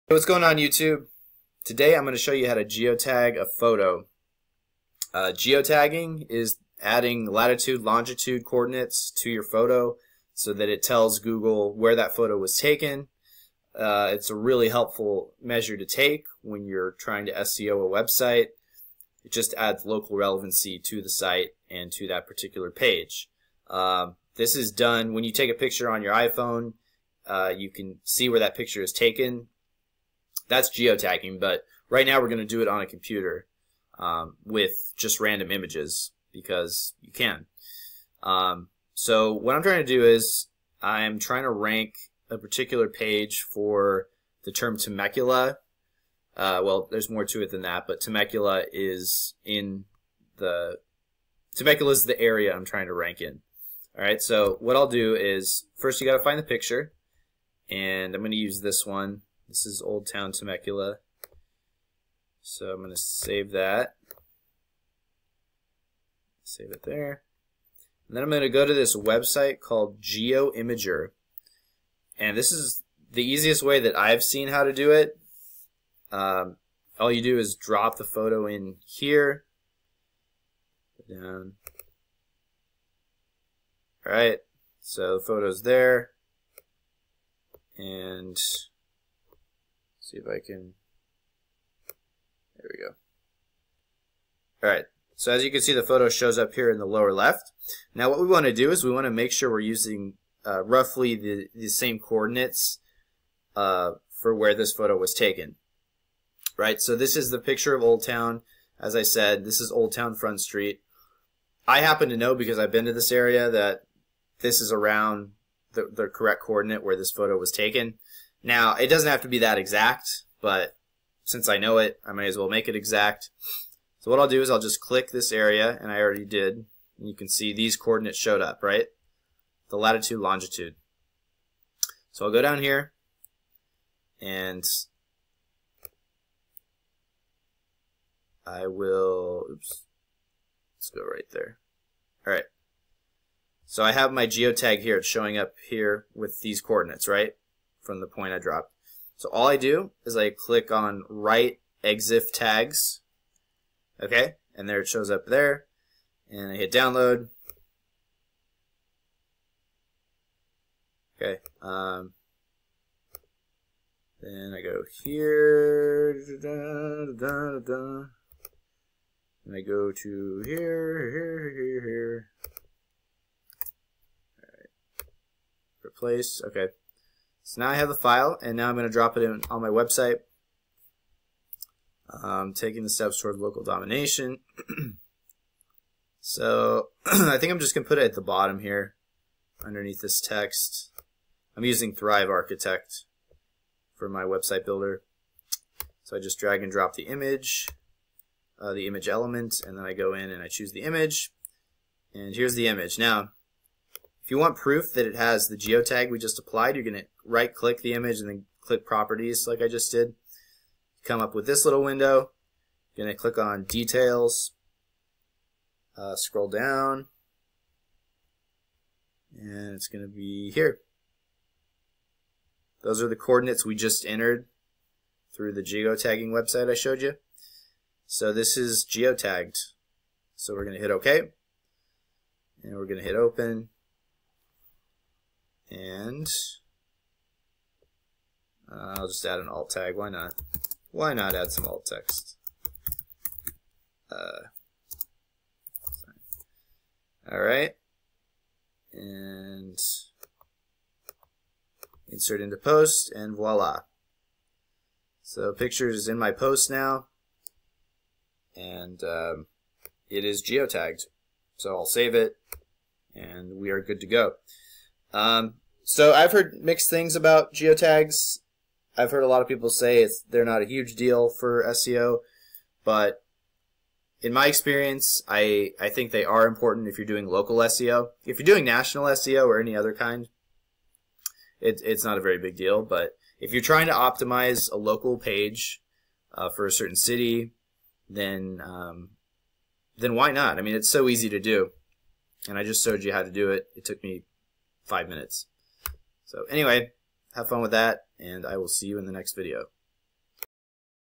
How to Geo Tag a Photo in 2018 (from Google Images)
In this video, we'll discuss how to geotag an image from Google and then post it to a Wordpress website.

for easy and FREE geotagging.